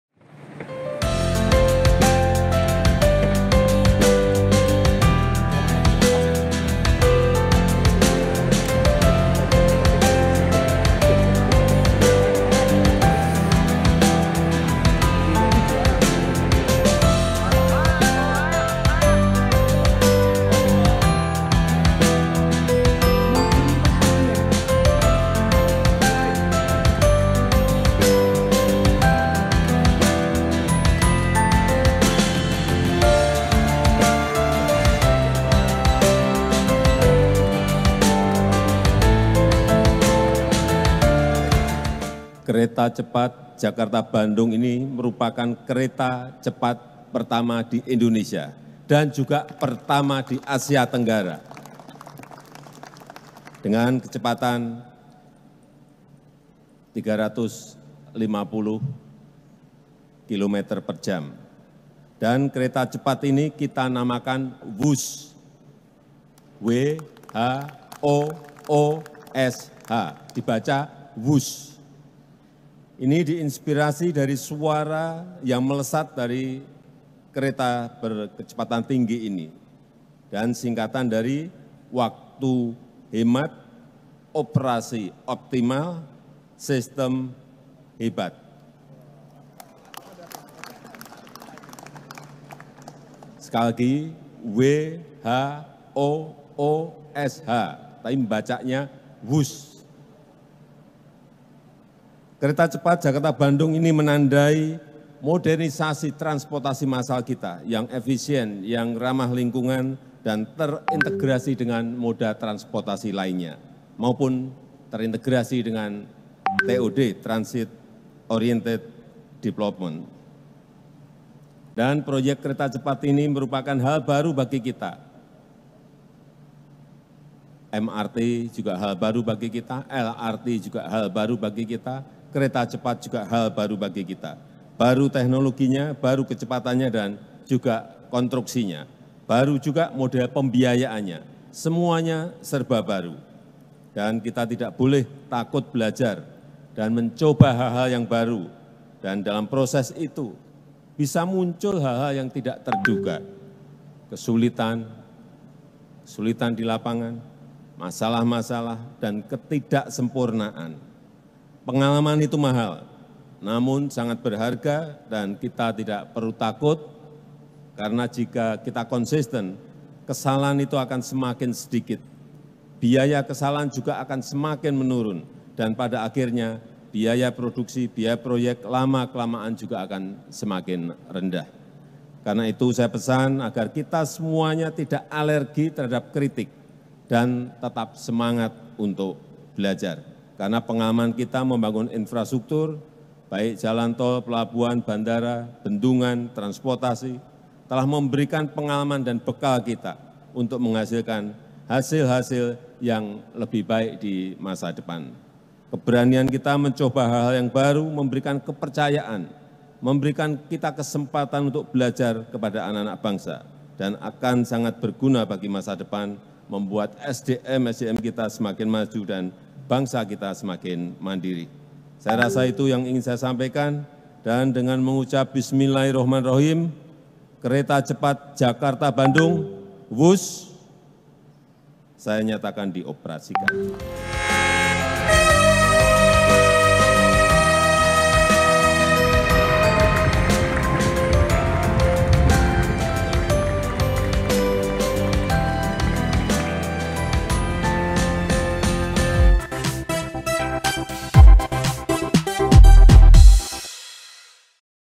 Perdana! Presiden Jokowi Resmikan Kereta Cepat Jakarta-Bandung
Presiden Jokowi meresmikan Kereta Cepat Jakarta-Bandung atau Whoosh. Peresmian dilakukan di Stasiun Halim, Senin (2/10) pagi.

Jokowi mengatakan Kereta Cepat ini akan dinamakan dan dibaca Whoosh. Kereta cepat diketahui menjadi yang pertama di Indonesia dan Asia Tenggara.

Jokowi menjelaskan nama Whoosh terinspirasi dari suara kecepatan dan memiliki singkatan waktu hemat operasi optimal sistem hebat (Whoosh).

Reporter: Raka Dwi Novianto
Produser: Akira Aulia W